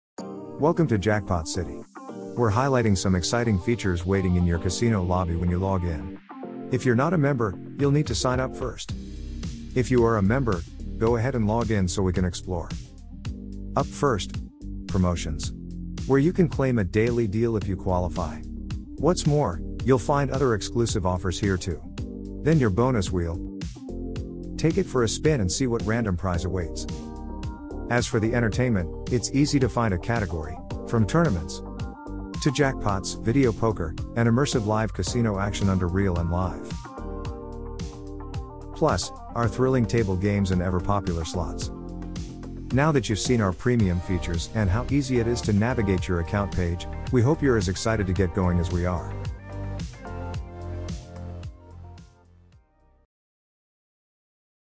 Enjoy Thrilling Features Available In the JackpotCity Casino Lobby | 18+
Find out about incredible deals that could potentially be waiting for you in the JackpotCity Casino Lobby! From thrilling Tournament games, daily promotions and spinning your Bonus Wheel - Enjoy all JackpotCity Casino has to offer.

We support Responsible Gaming. 18+ only.